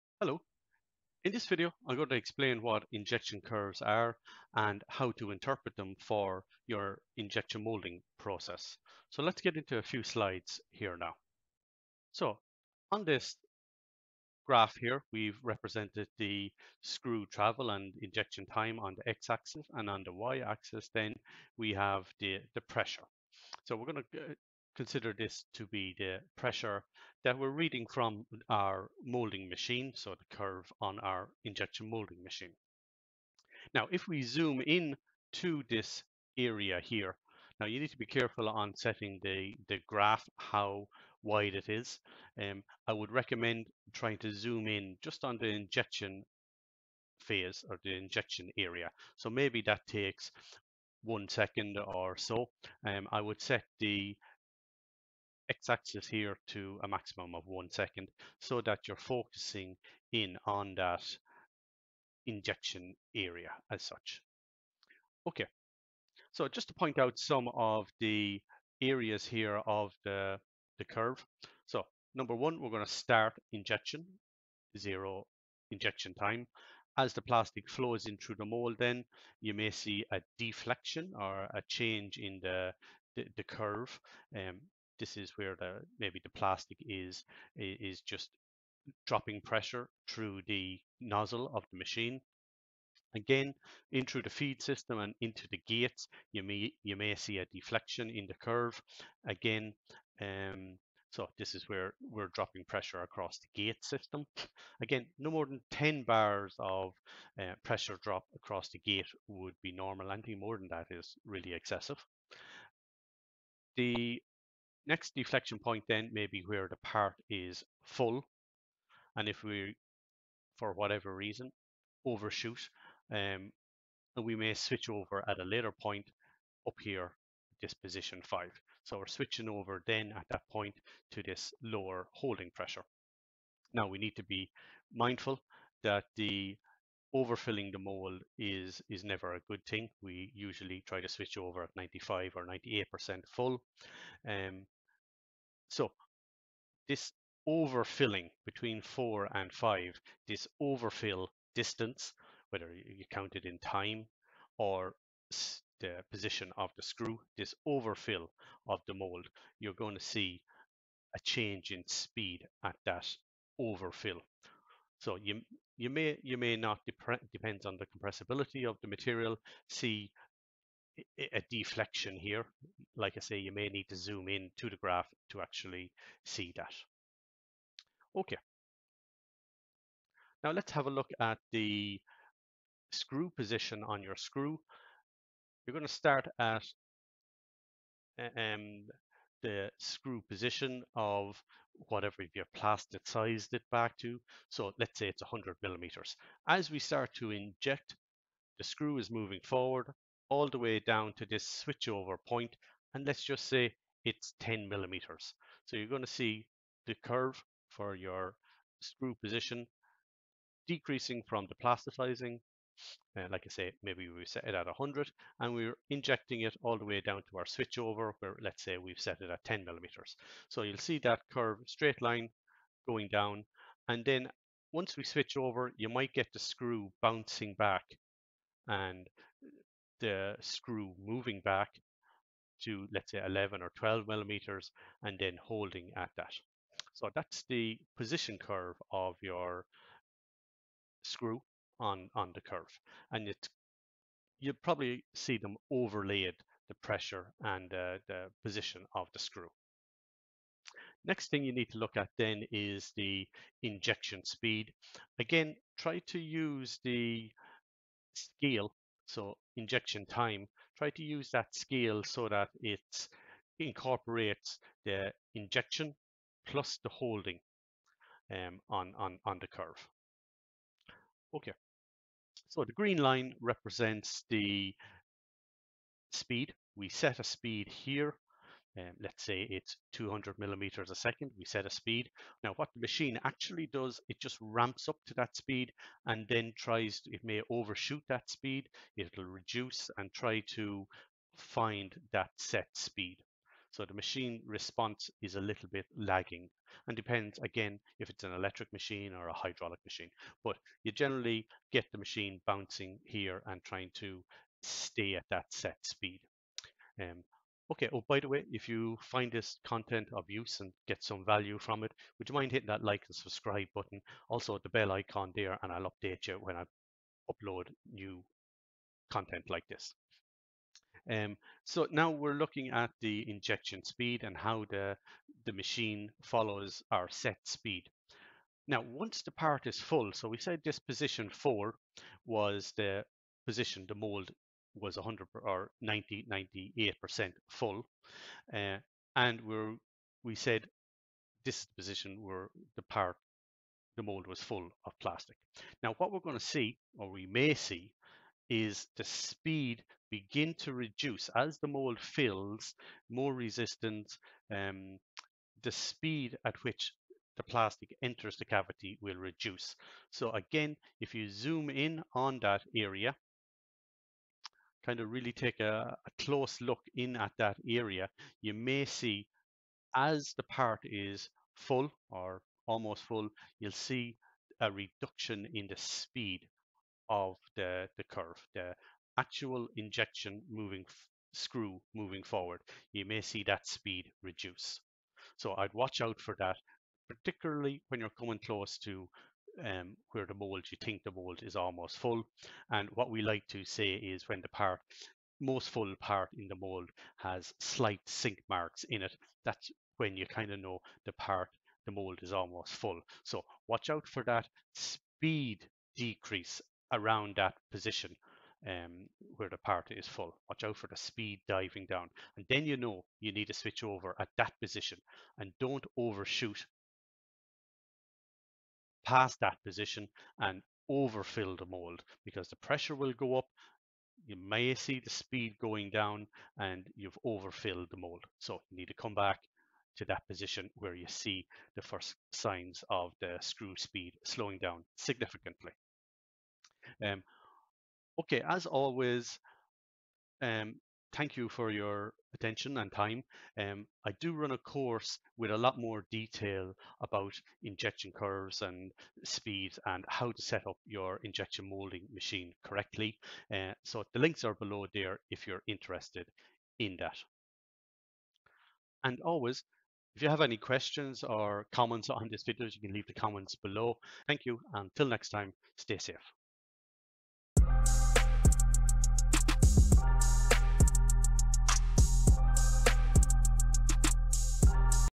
Injection molding - Injection curves explained
This video explains how to understand injection curves on a molding machine.